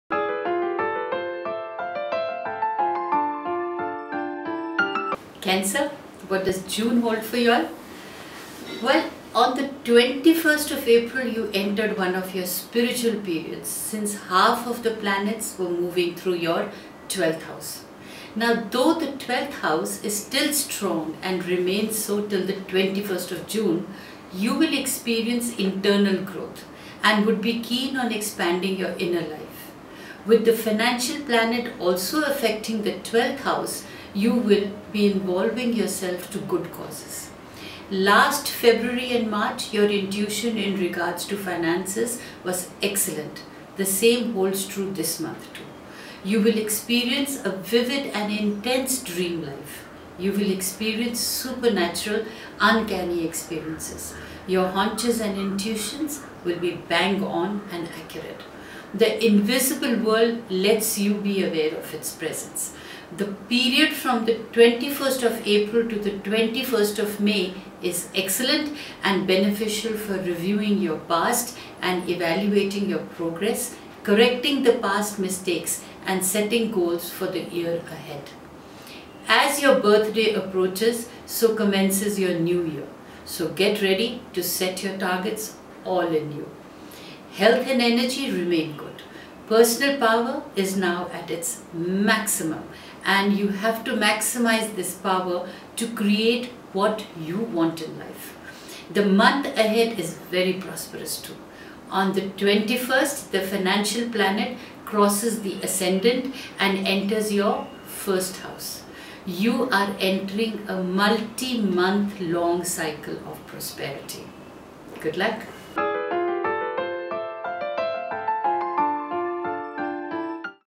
Cancer Horoscope: Jun 2015 Predictions
21st of April you entered one of your spiritual periods, since half of the planets were moving through your 12th house. Though the 12th house is still strong and remains so till the 21st, you will experience internal growth and would be keen on expanding your inner life. With the financial planet also affecting the 12th house you will involving yourself to good causes.

Last February and March your intuition in regards to finances was excellent the same holds true this month too. You will experience a vivid and intense dream life. You will experience supernatural uncanny experiences. Your haunches and intuitions will be bang on and accurate. The invisible world lets you be aware of its presence.

The period from 21st of April to the 21st of May is excellent and beneficial for reviewing your past and evaluating your progress correcting past mistakes and setting goals fro the year ahead. As your birthday approaches so commences your new year, so get ready to set your targets all anew.

Health and energy remain good personal power is now at its maximize this power to create what you want in life. The month ahead is very prosperous too. On the 21st the financial planet crosses the ascendant and enters your 1st house. You are entering a multimonth long cycle of prosperity.

God Bless
Dolly Manghat